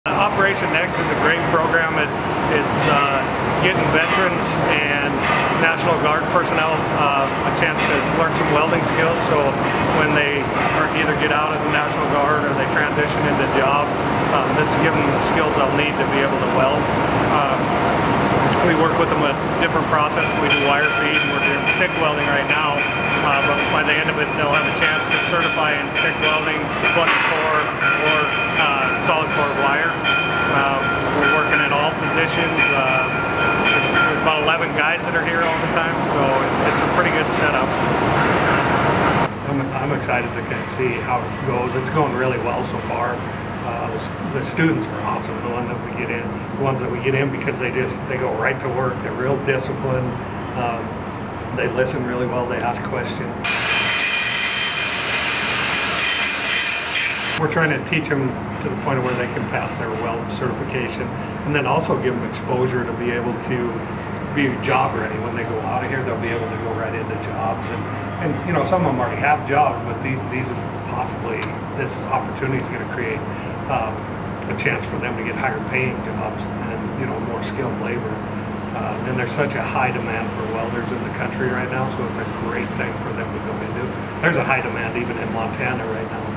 Operation Next at Highlands College in Butte
Veterans and members of the National Guard are diligently working away a Saturday as they learn new skills at Highlands College in Butte.